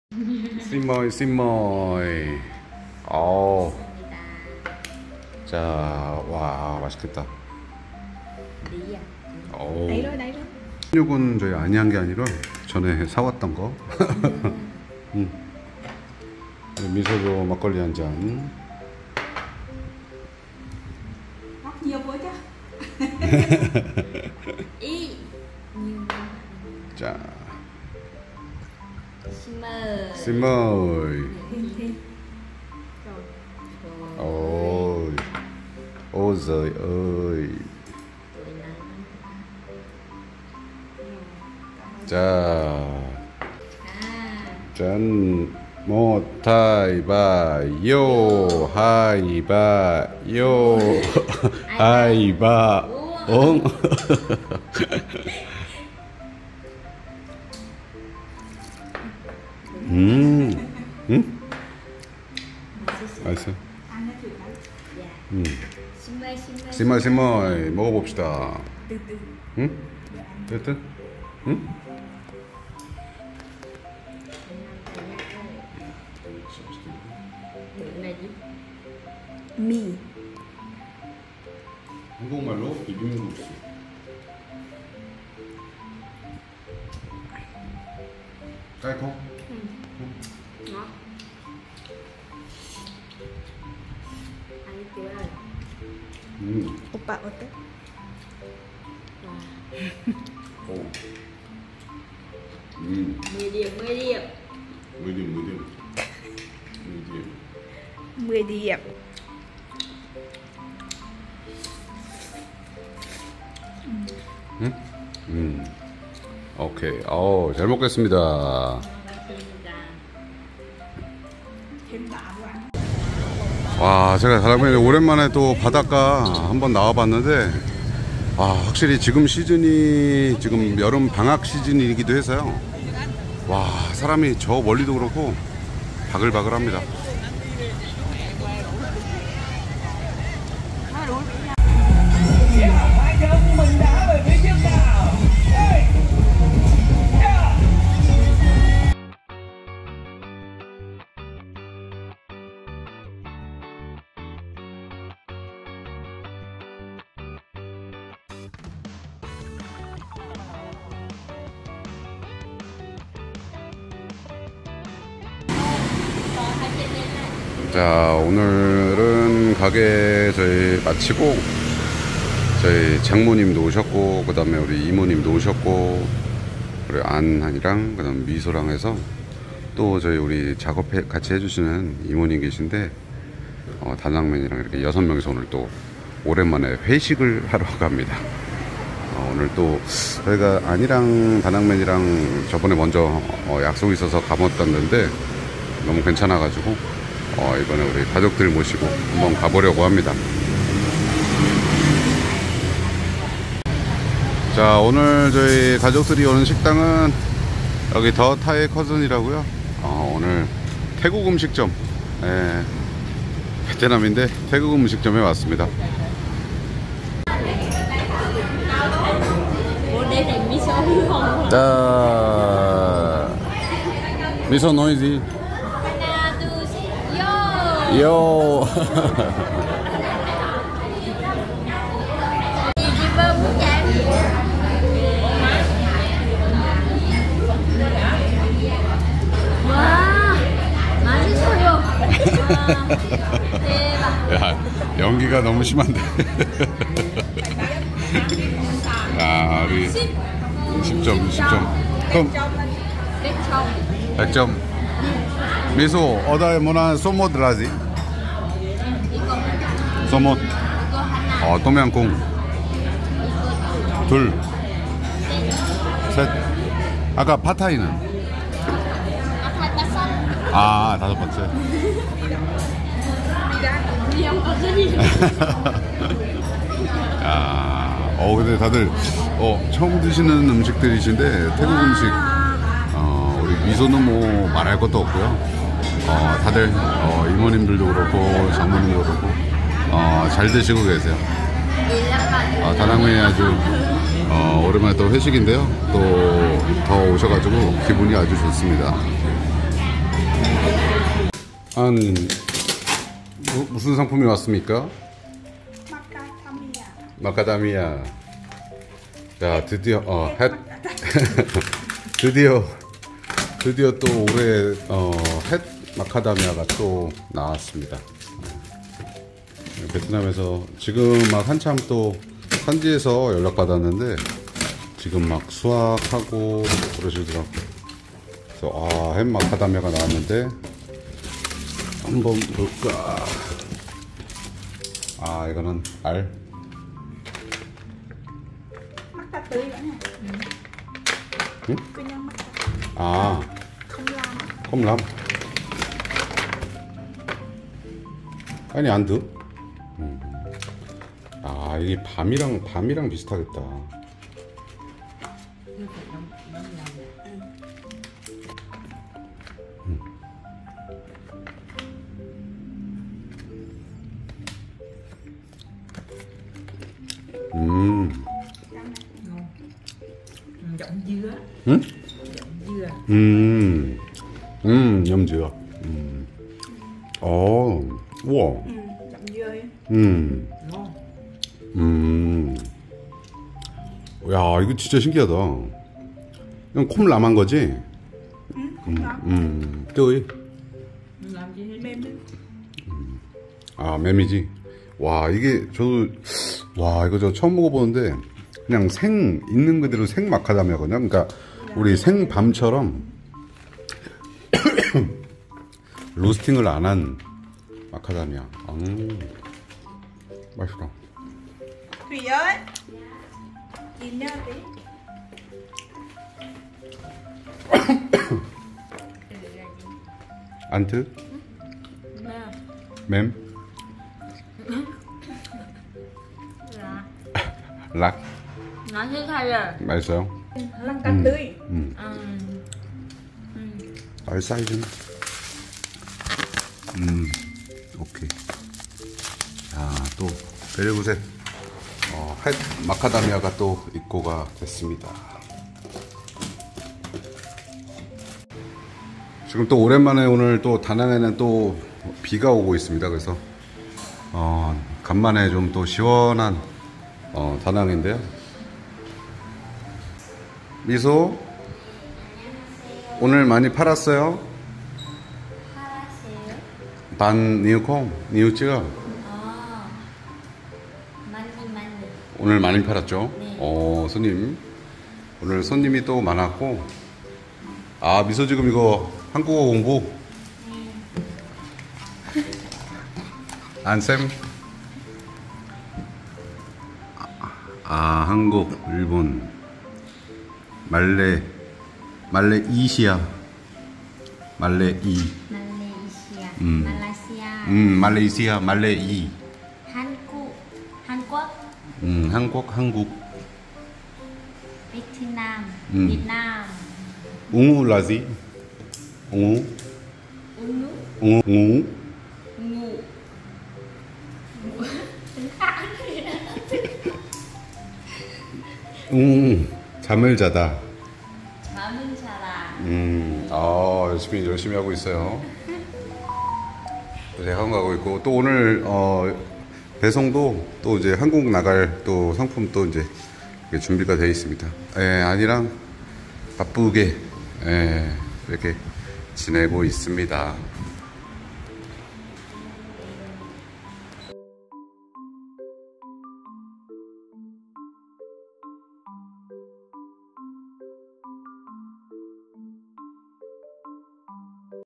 태국 음식 처음 먹어보는 베트남 가족 그리고 햇 마카다미아[국제커플]
안녕하세요 다낭맨입니다

다낭베리굿 및 여행관련 문의는 이곳으로 연락주세요

감사합니다^^